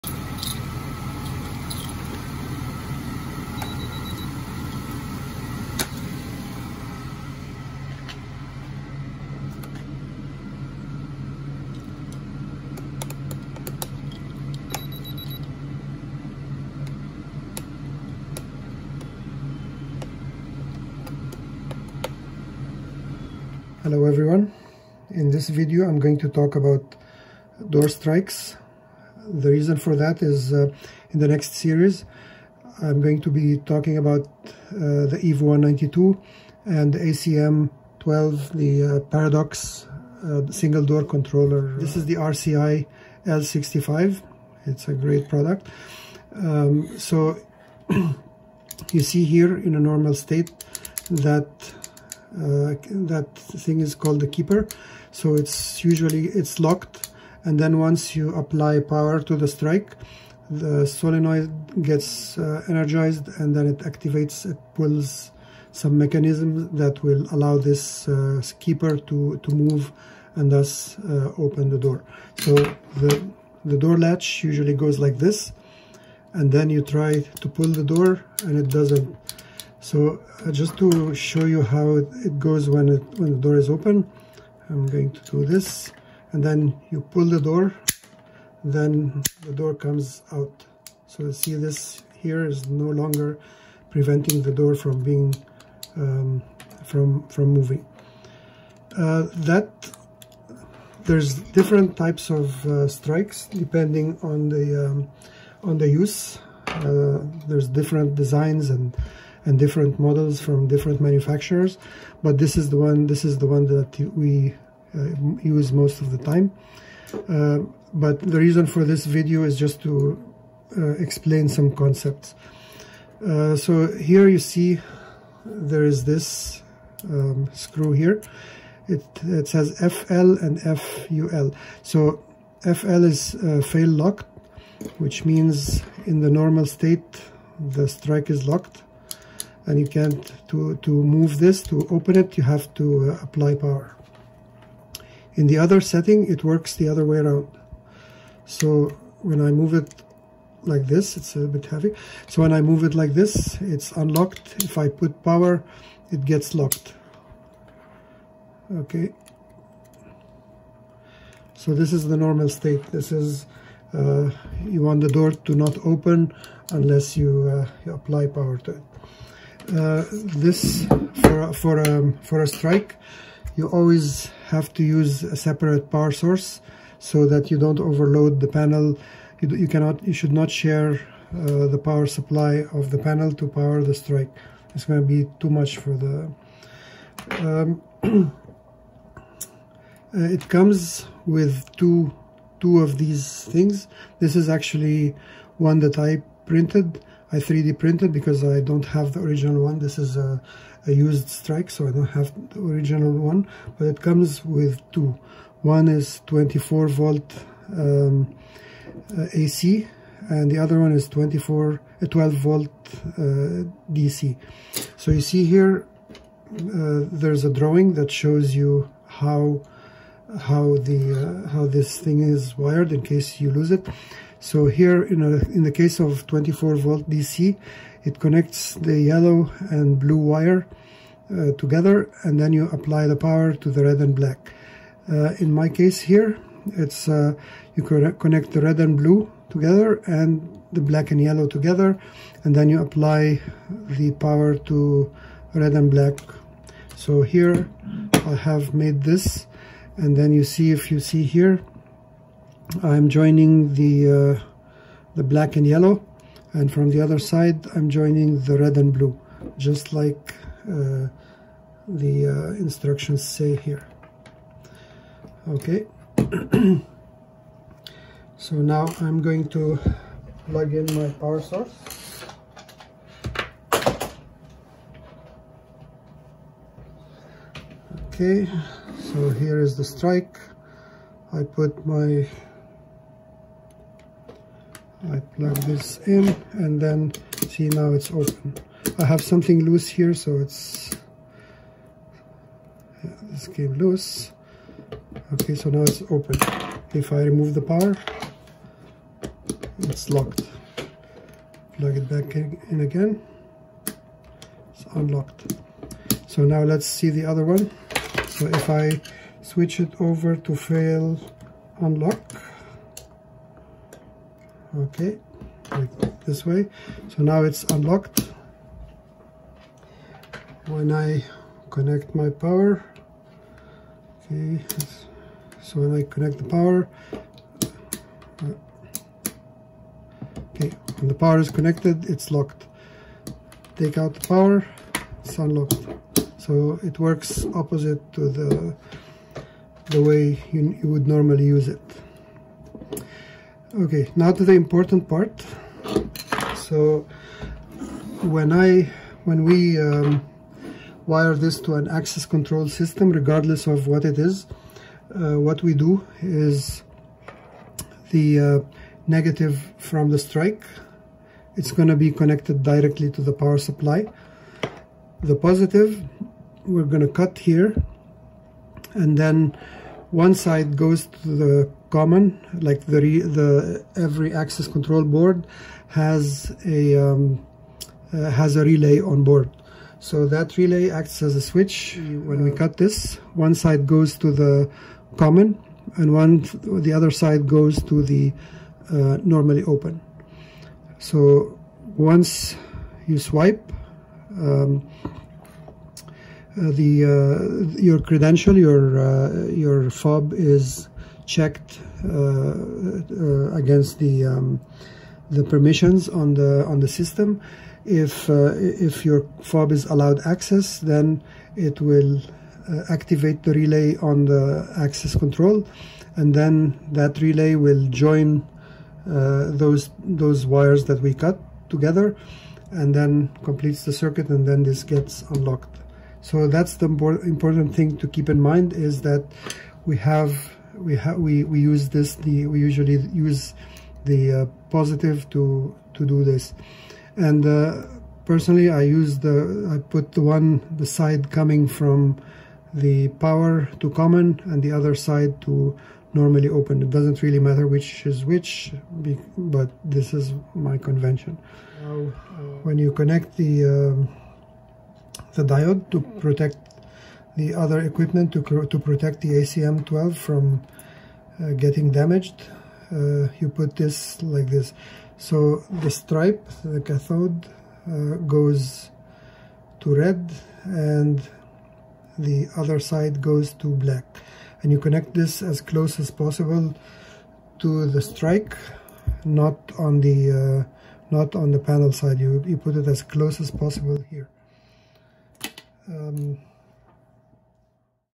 For Installers: Access Control Basics – Understanding Electric Door Strikes
Access control concepts for beginners explained clearly. Learn what electric door strikes are, how they work, and what every installer needs to know before setting up an access control system.